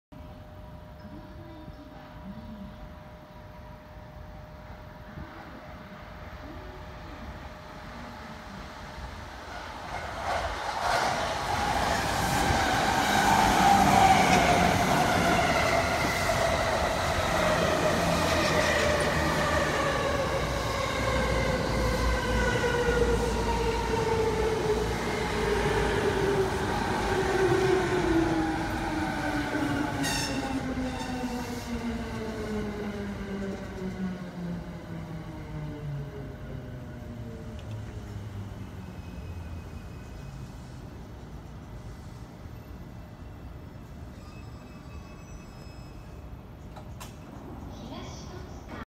JR東戸塚駅下り2番線に、下り15(4+11)両編成普通久里浜行き E217系2083SY-45が到着！
JR東戸塚駅2番線ホーム保土ヶ谷駅側から撮影！
保土ヶ谷駅を発車して走行中の、下り15(4+11)両編成横須賀線普通久里浜行き E217系2083SY-45の電車が、東戸塚駅下り2番線ホームに到着！